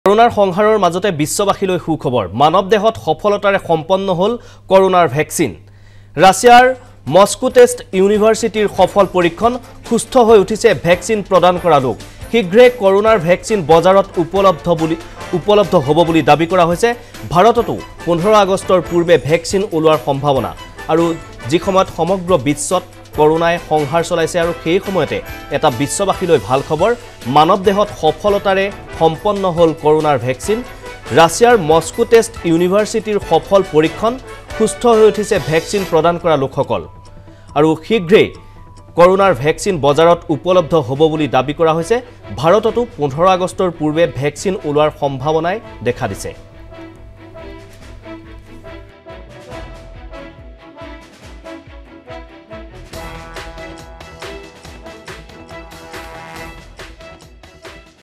Russia successfully completes human trials of coronavirus vaccine
মানৱ দেহত সফলতাৰে সম্পন্ন কৰোনাৰ ভেকচিন, ৰাছিয়াৰ মস্কো ষ্টেট ইউনিভাৰচিটীৰ সফল পৰীক্ষণ